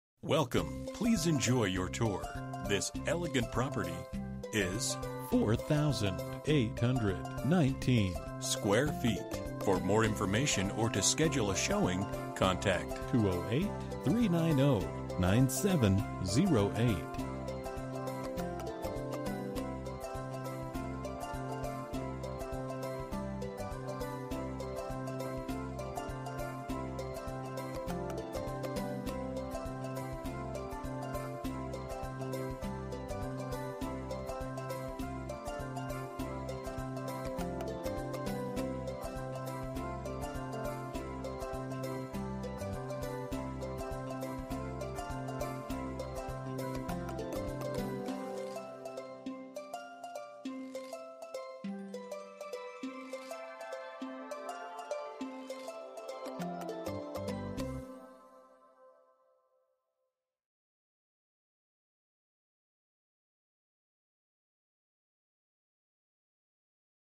Homes for sale - 10 W Judicial, Blackfoot, ID 83221
Great investment at a return of 7.46 CAP. Guaranteed corporate lease to HUB International.  Single tenant, no vacancy. Assumable lease expiring 2020 with 5 yr. extension  COL annual increases. Very visible on main thru fare Long time of community identity for insurance company. Current tenant been at this location over 12 yrs.

Bedrooms: 0
Bathrooms: 0.00
Square Feet: 4819
Price: $900,000